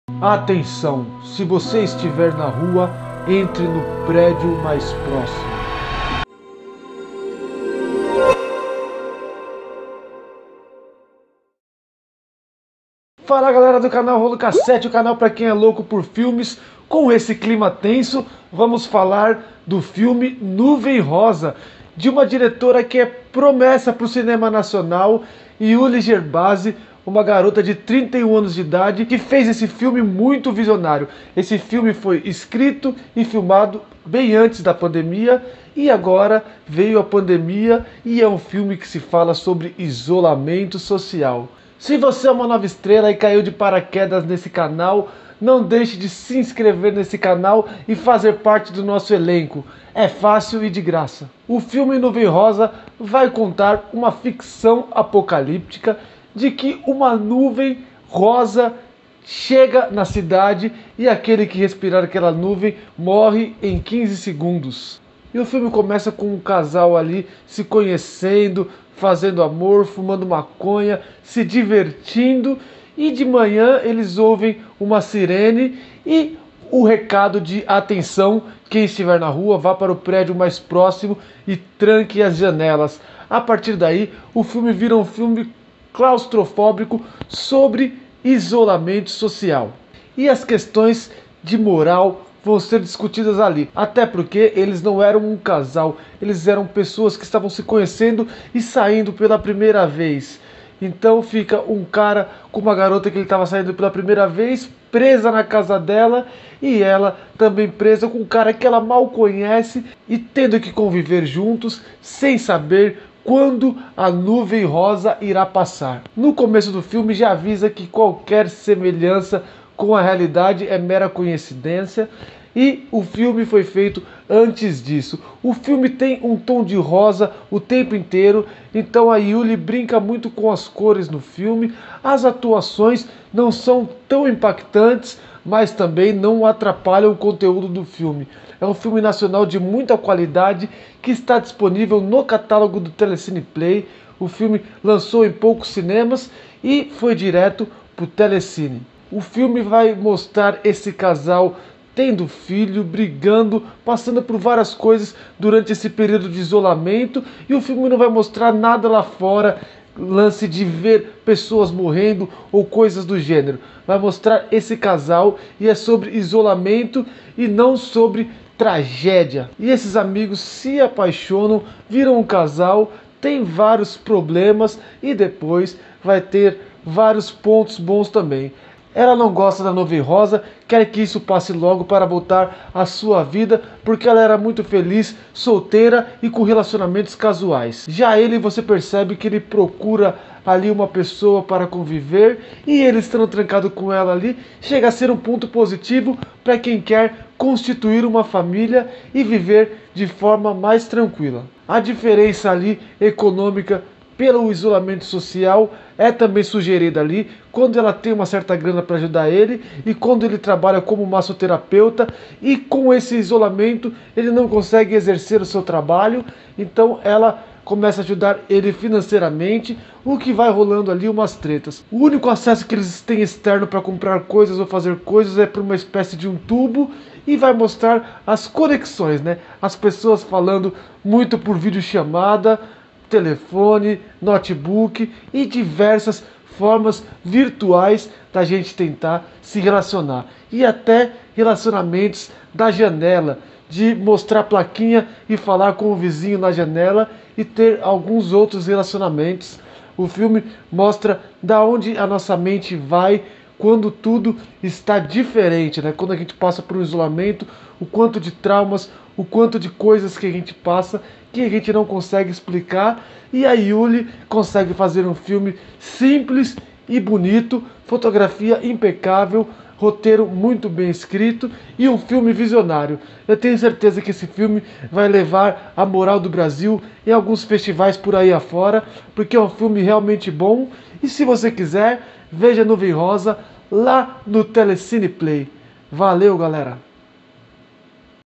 Nuvem rosa - O filme que previu a pandemia e o isolamento social
O filme nacional Nuvem rosa de Iuli Gerbase previu o que passaríamos durante a pandemia do coronavírus com o isolamento social e também discute relacionamentos e traumas causados pela privação da liberdade, o filme está disponível no telecine play para quem quiser conferir.

TWEETER: @k7rolo